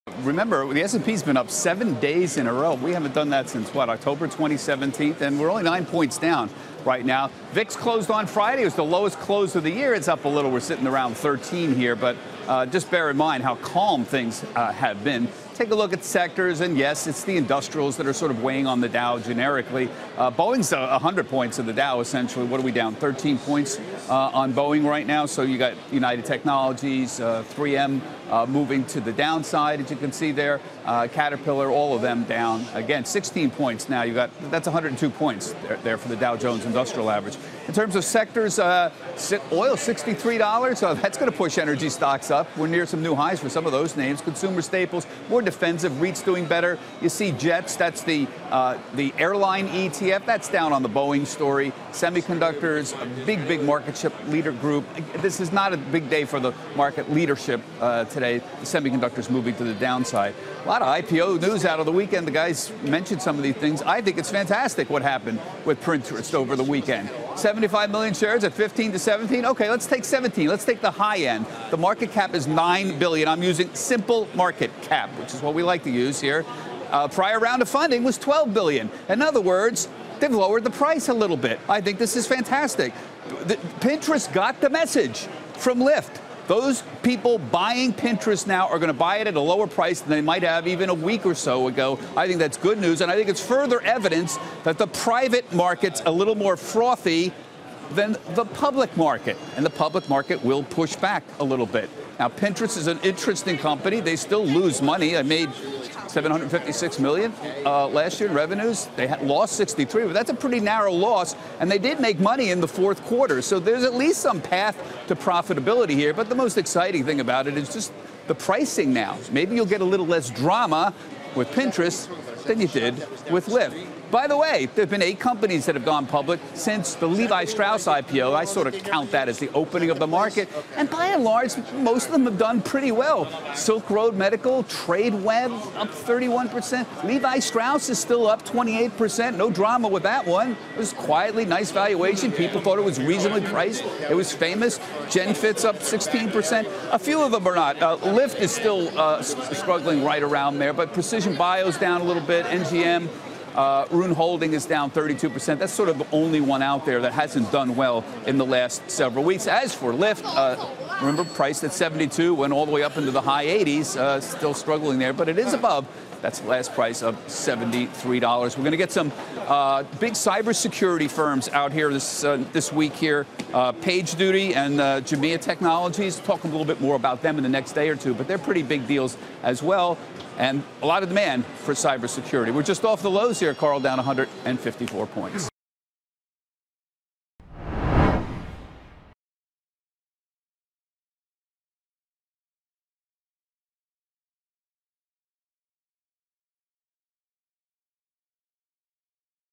Markets open lower as industrials weigh on the Dow
CNBC's Bob Pisani looks ahead at the day's market action.